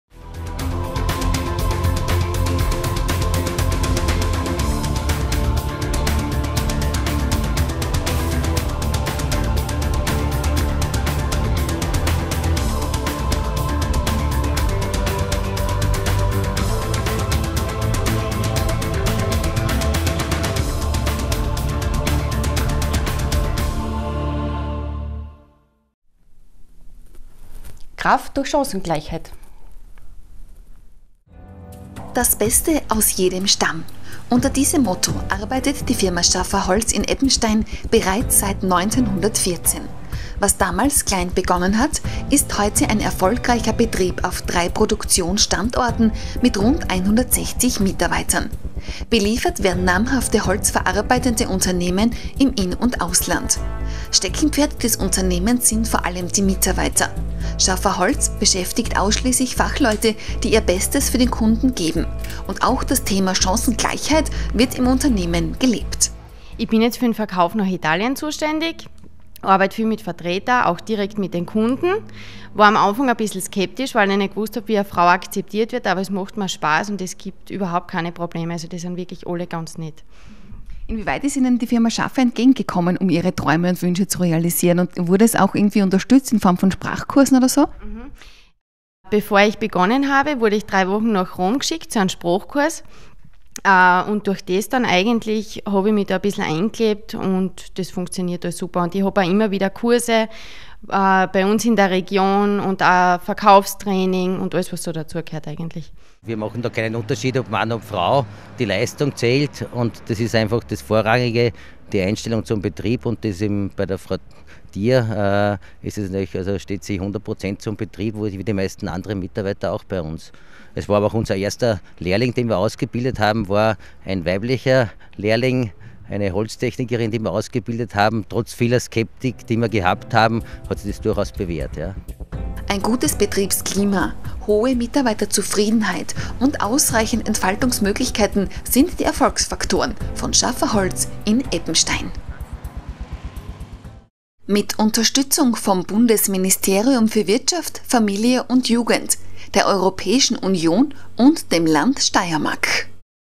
Schaffer Holz
Die Familie Schaffer betreibt nunmehr seit 1914 in 3. Generation das Sägewerk SCHAFFER HOLZ.
Der Name SCHAFFER HOLZ wird in der Holzindustrie mit Starkholz verbunden. Gotthard Schaffer setzte in den 90-er Jahren einen wichtigen Schritt zur Absicherung der Zukunft des Unternehmens, als er beschloss, sich auf die Verarbeitung von Starkholz zu spezialisieren. Seit mehr als 20 Jahren führt nun sein Sohn Hans Schaffer das Familienunternehmen. In dieser Zeit hat sich der Einschnitt von 45.000 fm auf 140.000 fm gesteigert.
Neben Starkholz hat man sich auf die österreichische Lärche spezialisiert. So wurden letztes Jahr etwa 25.000 cbm Lärchenschnittholz vermarktet. 1999 wurde das Tiroler Lamellierholzwerk TLH, heute SCHAFFER HOLZ Tirol, mit Sitz in Silz 100%ige Tochter von SCHAFFER HOLZ.

2008 übernahm SCHAFFER HOLZ die Mehrheitsbeteiligung an dem slowenischen Starkholzsägewerk. Heute beschäftigt SCHAFFER HOLZ rund 160 Mitarbeiter an drei Produktionsstandorten und beliefert namhafte holzverarbeitende Unternehmen im In- und Ausland.

Die Mitarbeiter bezeichnet Hans Schaffer als sein größtes Kapital. Im Betrieb wird sehr viel Wert auf ein gutes Betriebsklima und Mitarbeiterzufriedenheit gelegt. So ergab die letzte Mitarbeiterbefragung, dass 95 % der Beschäftigten in keinem anderen Betrieb arbeiten möchten.

Die große Herausforderung in Zeiten der Finanzkrise sieht man bei SCHAFFER HOLZ in der Absicherung der derzeitigen Marktposition sowie in der Eroberung neuer Märkte.